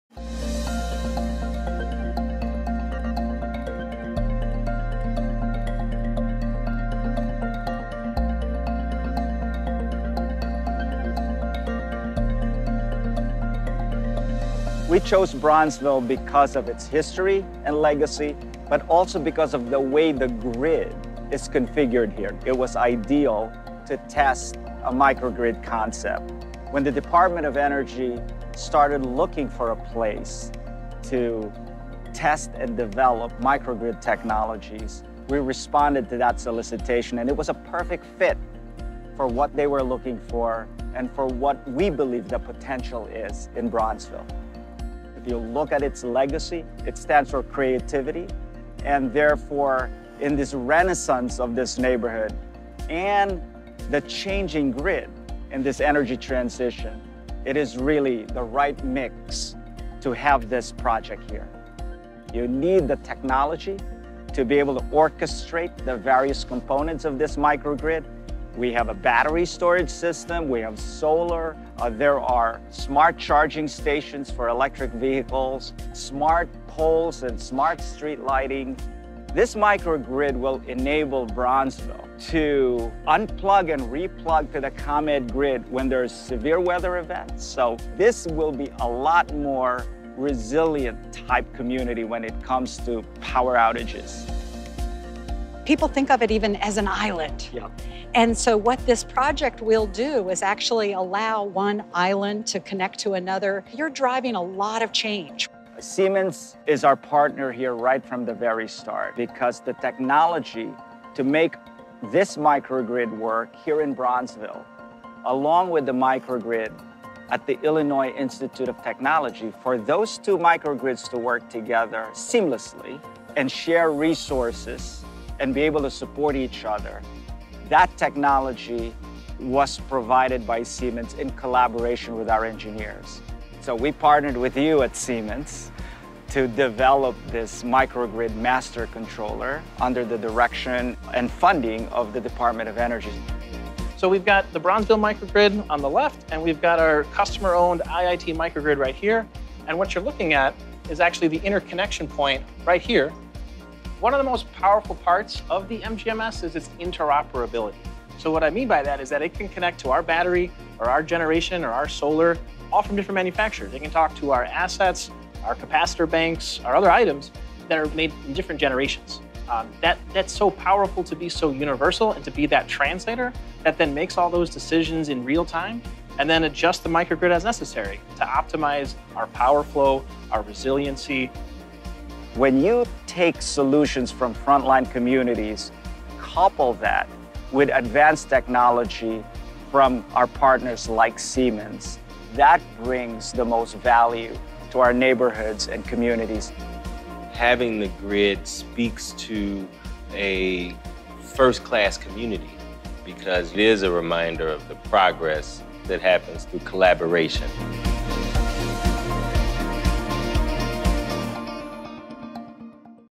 Siemens Customer Story - ComEd/Bronzeville Microgrid
Siemens USA CEO, Barbara Humpton, visits with Siemens customer, ComEd to tour their Bronzeville microgrid cluster outside of Chicago. Barbara speaks with ComEd CEO, Gil Quiniones, about their partnership and how ComEd is using Siemens microgrid management system software (MGMS) to run the first utility-operated microgrid cluster and redefine how utilities can use distributed grid architecture to provide reliable, resilient, and efficient energy.